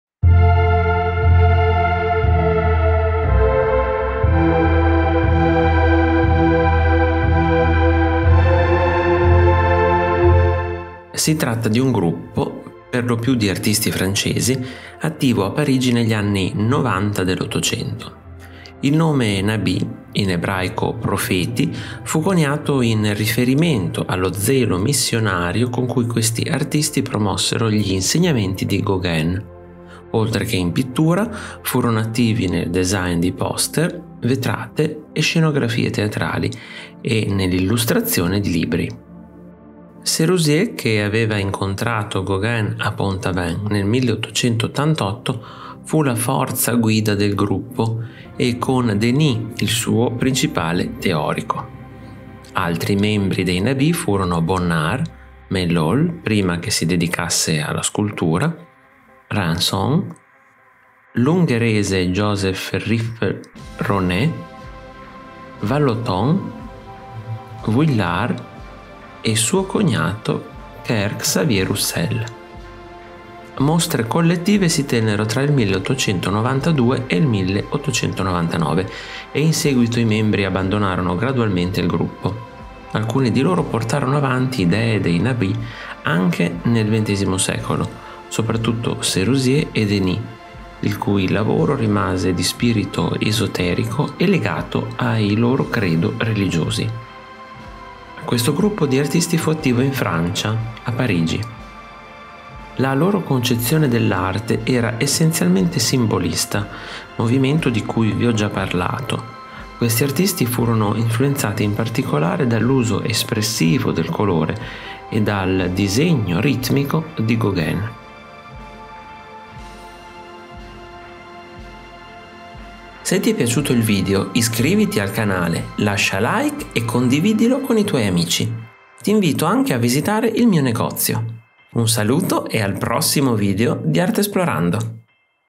Nabis
Le Artesplorazioni sono una serie di video che vi guideranno tra i movimenti e i temi della storia dell'arte, rispondendo alle 5 domande: cosa, chi, dove, quando e perché. 😊 Oggi parliamo dei Nabis! ▼▼CONTINUA SOTTO▼▼

Si tratta di un gruppo, perlopiù di artisti francesi, attivo a Parigi negli anni Novanta dell’Ottocento. Il nome Nabis, in ebraico profeti, fu coniato in riferimento allo zelo missionario con cui questi artisti promossero gli insegnamenti di Gauguin. Oltre che in pittura, furono attivi nel design di poster, vetrate e scenografie teatrali e nell’illustrazione di libri.
Serusier, che aveva incontrato Gauguin a Pont-Aven nel 1888, fu la forza guida del gruppo e con Denis il suo principale teorico. Altri membri dei Nabis furono Bonnard, Maillol, prima che si dedicasse alla scultura, Ranson, l’ungherese Josef Rippl-Ronai, Vallotton, Vuillard e suo cognato Ker-Xavier Roussel.

LETTURE CONSIGLIATE
➡ I Nabis. Ediz. illustrata